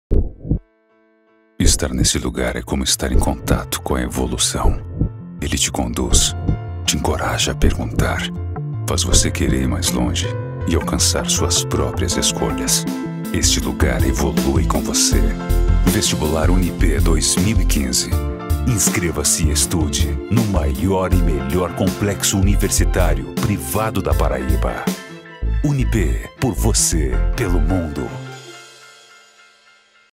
Unipê | Campanha "Vestibular 2015" | 2014
CLIENTE: Unipê
CAMPANHA: Vestibular 2015
COORDENADOR DE CRIAÇÃO: Masao Yogi
DIRETOR DE ARTE: Rodi Cruz
REDATOR: Masao Yogi
RTVC: Leo Saraiva
ATENDIMENTO: Helton Nóbrega/ Mirella Monteiro
MÍDIA: Simony Batista/ Tarciano Pereira
PRODUTORA DE VÍDEO: Canário
PRODUTORA DE ÁUDIO: Tango
DIRETOR DE CENA: Rafael Monguilhott
PRODUÇÃO EXECUTIVA: Ana Carolina Silveira